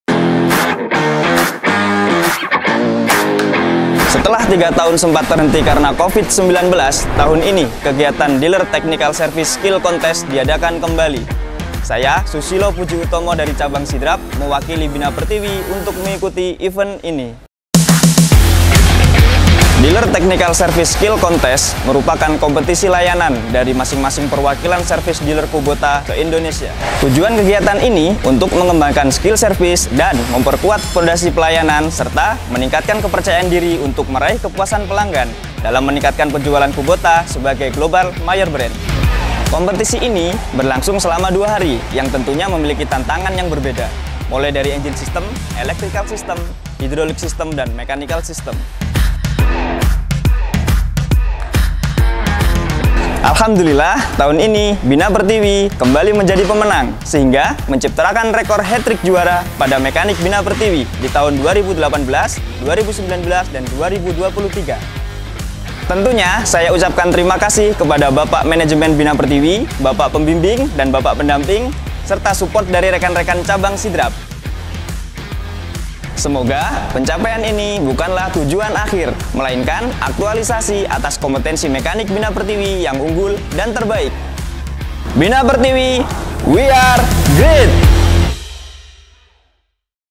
Susilo Puji Utomo, pemenang Dealer Technical Service Contest Kubota Machinery Indonesia 2023
Hai Great People!

Bina Pertiwi selalu berusaha memberikan yang terbaik untuk setiap stakeholder, termasuk dengan memberikan layanan support melalui manpower yang unggul dan berkompeten.

Susilo Puji Utomo, salah satu mekanik berprestasi yang berhasil meraih juara utama pada Dealer Technical Service Contest Kubota Machinery Indonesia 2023 yang berlangsung pada 26 - 27 Juli 2023. Kemenangan ini, membawa rekor baru untuk Bina Pertiwi karena berhasil menjadi pemenang selama tiga kali berturut-turut pada kompetisi ini.